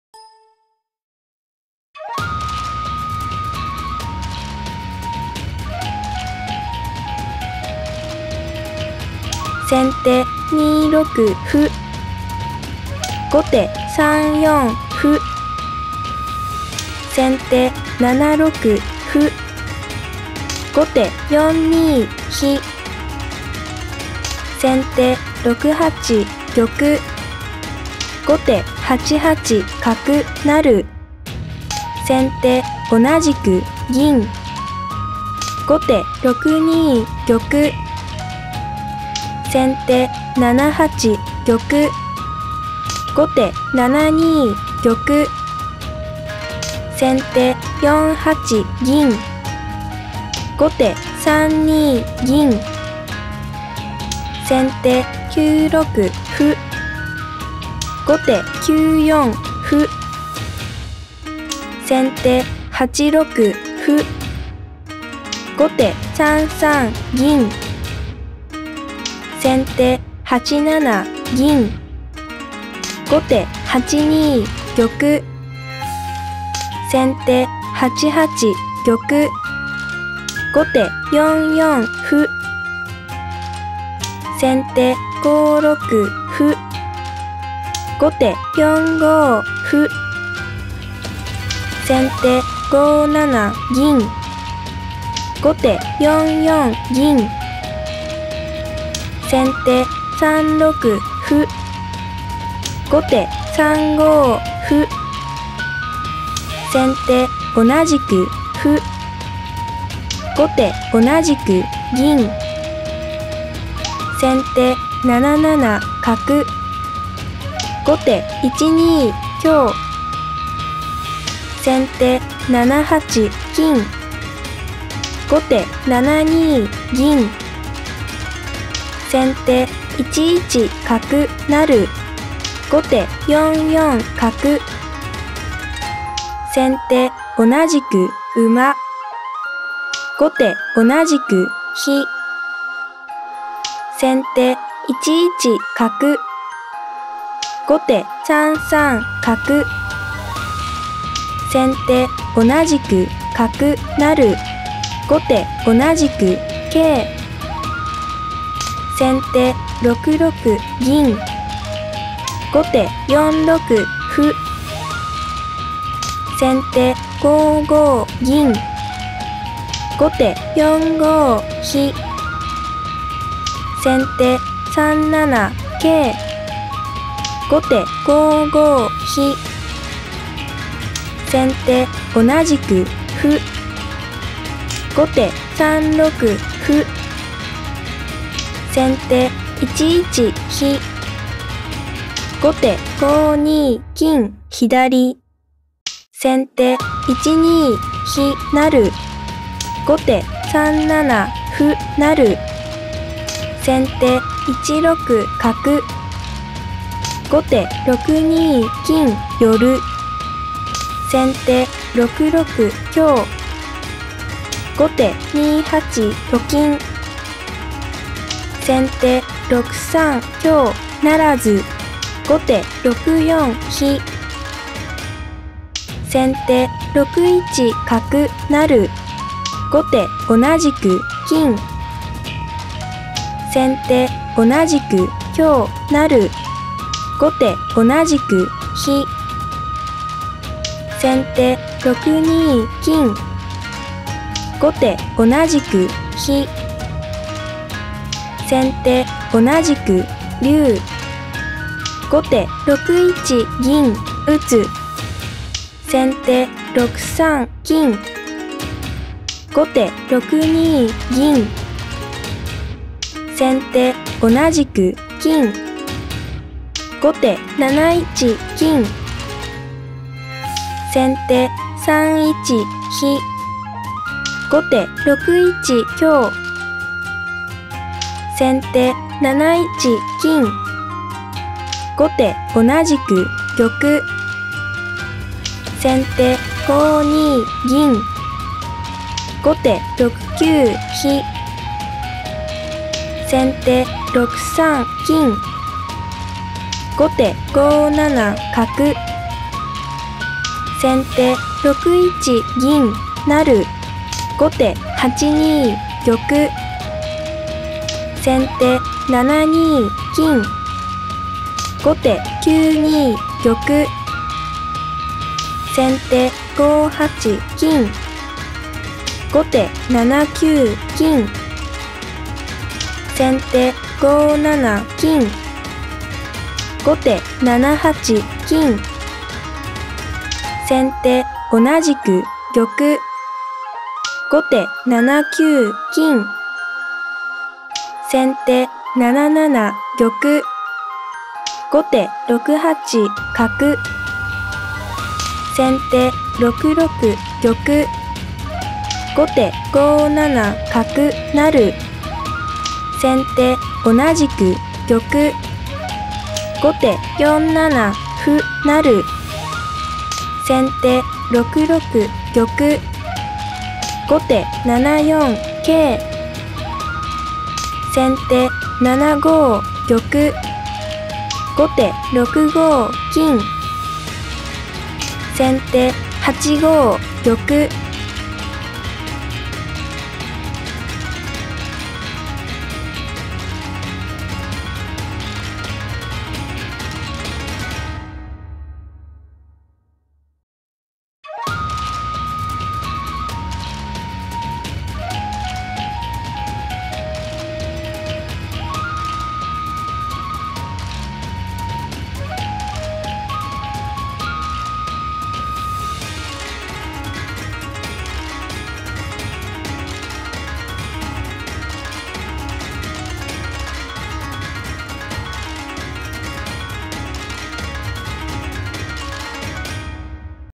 将棋 棋譜並べ ▲広瀬章人八段vs △鈴木大介九段 第30期竜王戦２組昇級者決定戦 第27回世界コンピュータ将棋選手権優勝「elmo」の棋譜解析 No.103
開始日時：2017/06/02 10:00
終了日時：2017/06/02 17:56
棋戦：第30期竜王戦２組昇級者決定戦
持ち時間：5時間
場所：東京将棋会館
手合割：平手
残り時間：先手：39分,後手：2時間50分
先手：広瀬章人八段
後手：鈴木大介九段
戦型：角交換四間飛車
備考：elmoの評価関数と定跡を使用しています。エンジンはやねうら王です。